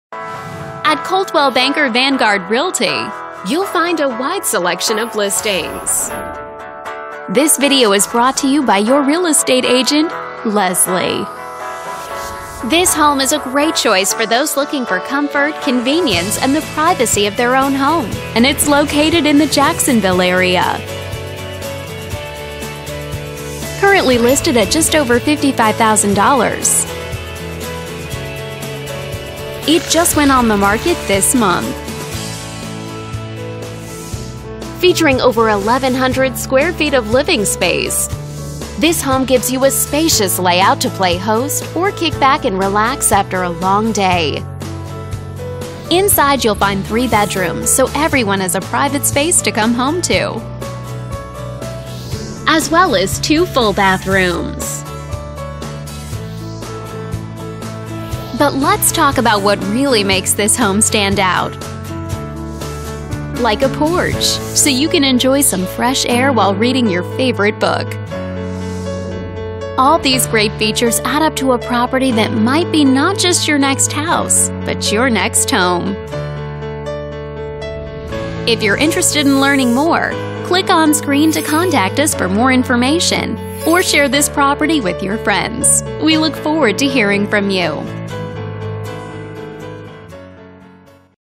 3701 SKYCREST
Looking for LOCATION, A BIG BACK YARD AND AFFORDABILITY - you just found it!  Welcome to this charming home with three bedrooms and two baths.  Separate areas for living and dining but with an open feel.  A large screened porch for relaxing or entertaining.  Close to shopping, dining, Tinsel Town and St. Johns Town Center.  Commuting made easy with Southside Blvd, Beach Blvd, Altantic Blvd, JTB, and I-295 close by.  Welcome home.